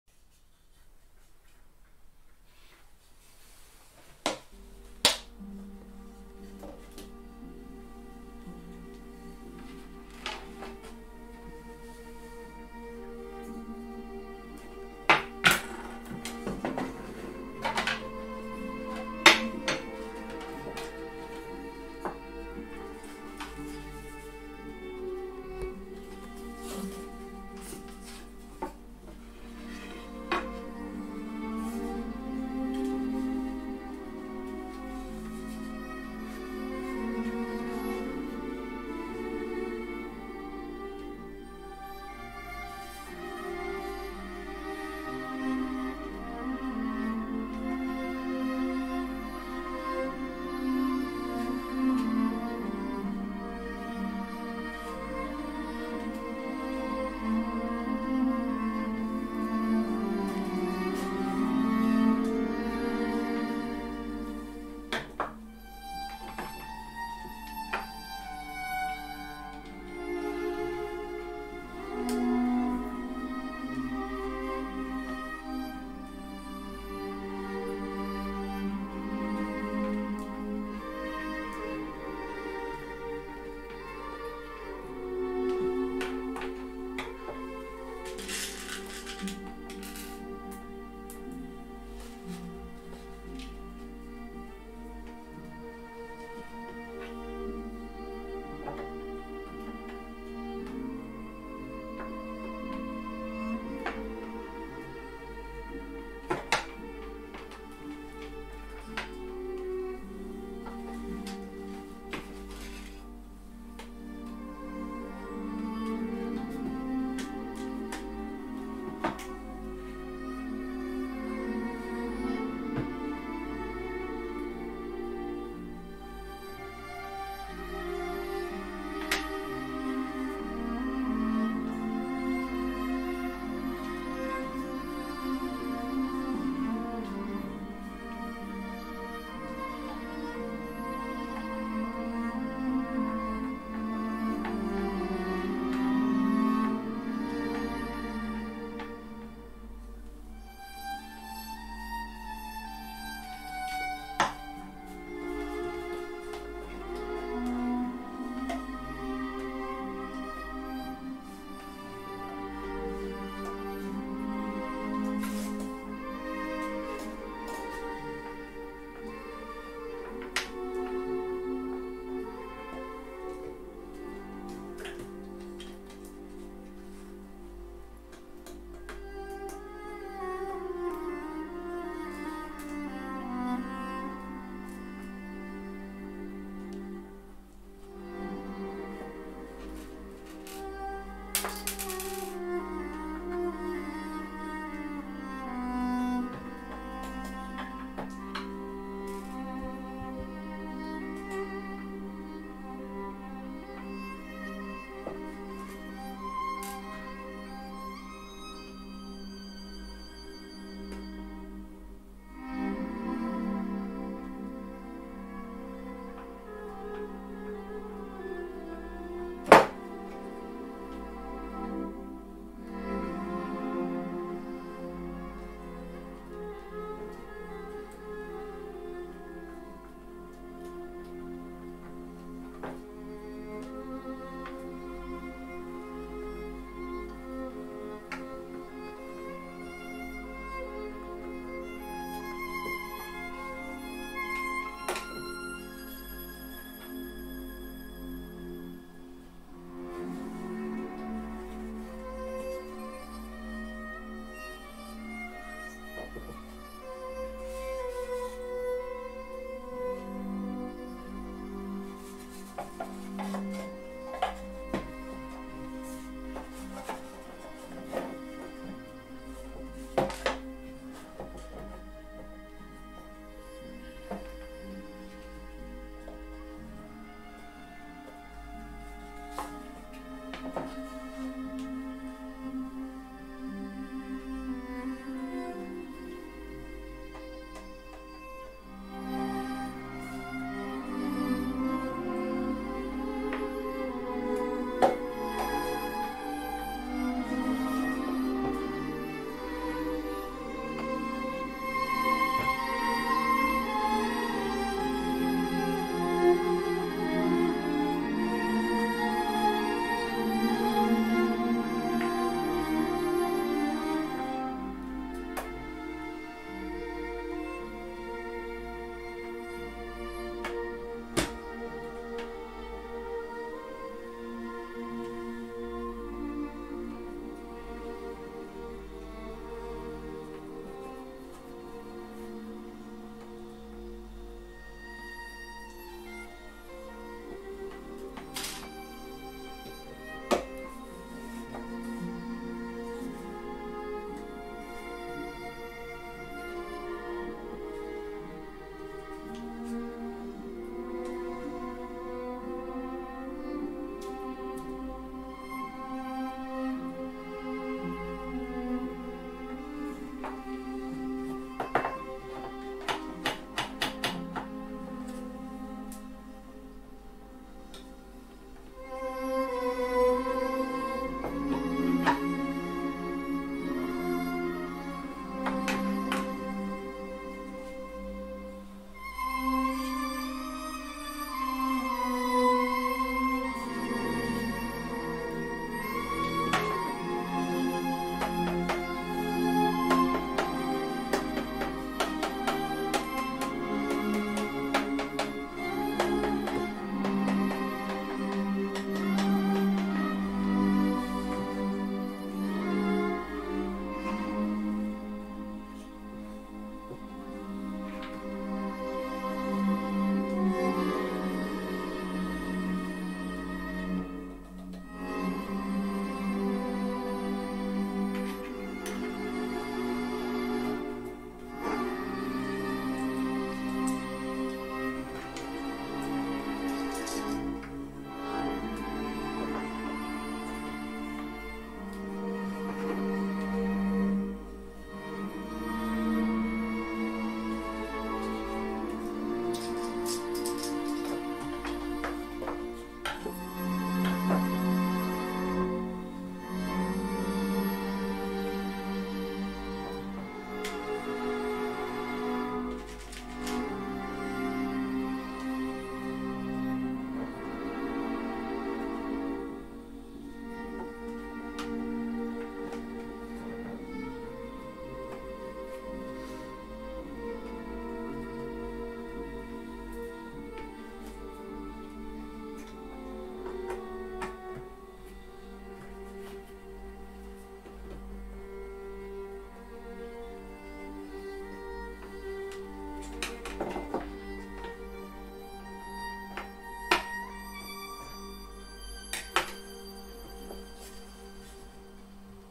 How to make a folk violin with your own hands from waste materials / fiberglass soundboard / Part 3
In this and the following videos I will show you how to make a folk violin from fiberglass and scrap wood.